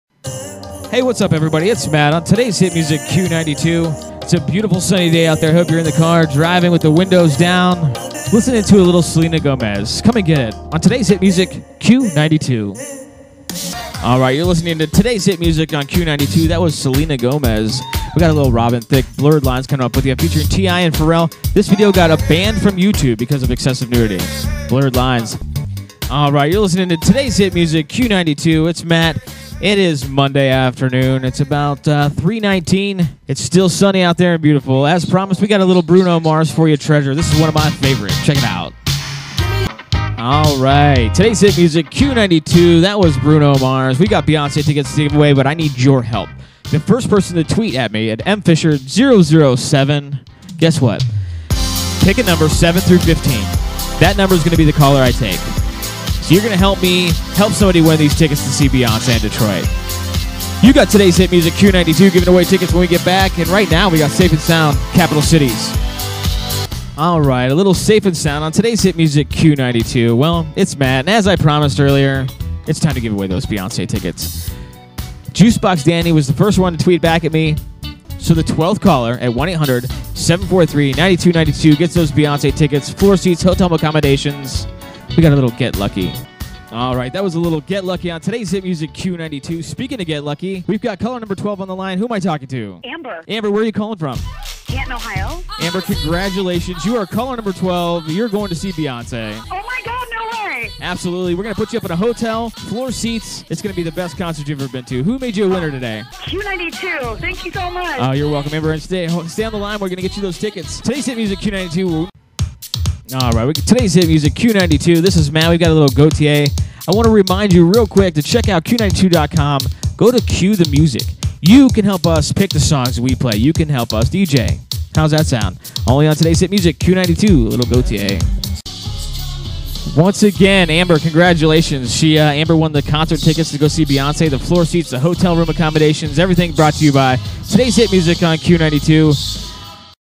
Demo Q92 Side A
A demo I made for WDJQ 92.5 "Today's Hit Music"